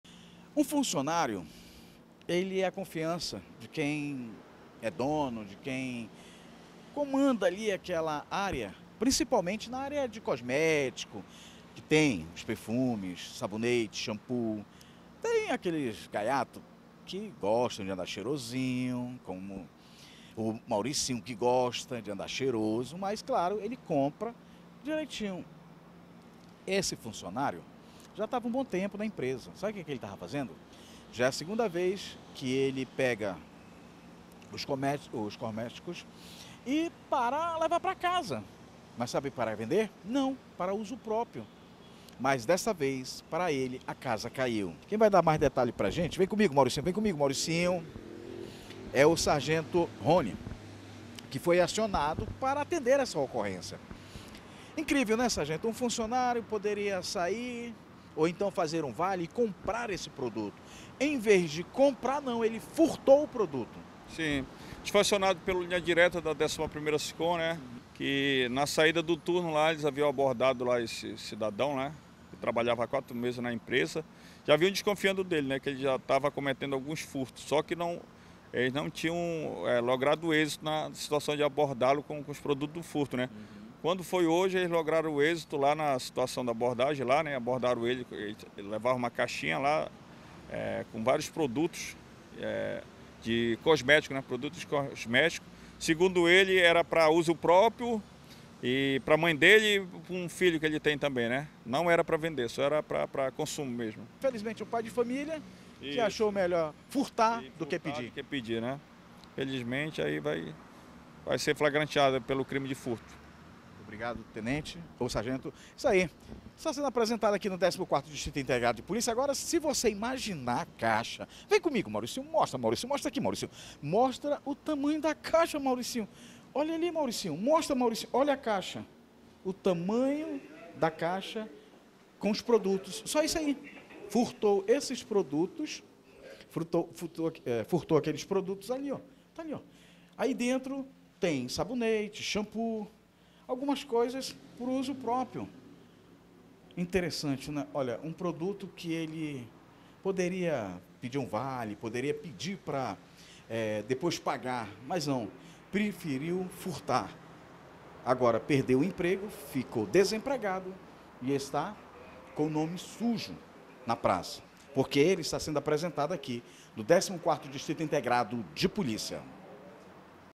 Homem é preso por furtar cosméticos do estabelecimento onde trabalhava
Funcionário é preso por furtar cosméticos em empresa de Manaus